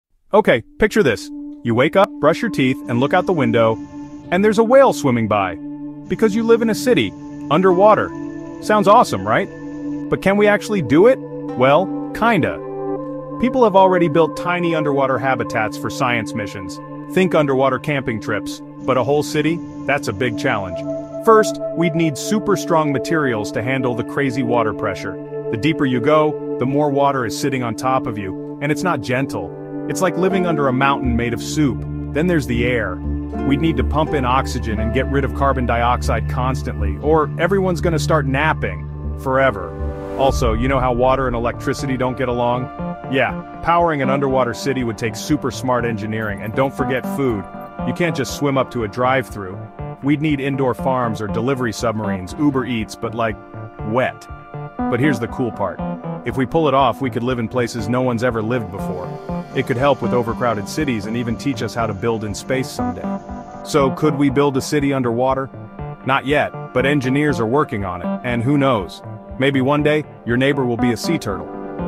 Could We Ever Build a City Underwater?
Imagine waking up to a whale swimming by your window in an underwater city! 🌊 In this video, we explore the fascinating concept of living beneath the waves. Dive deep into the incredible challenges that come with building an underwater metropolis, from pressure-resistant materials to managing air quality and energy. Discover how tiny underwater habitats are paving the way for future innovations, including sustainable food sources and advanced engineering solutions. Could this groundbreaking idea help alleviate urban overcrowding and even prepare us for living in outer space? Join us as we venture into the depths and dream about a future where your neighbor might just be a sea turtle! 🐢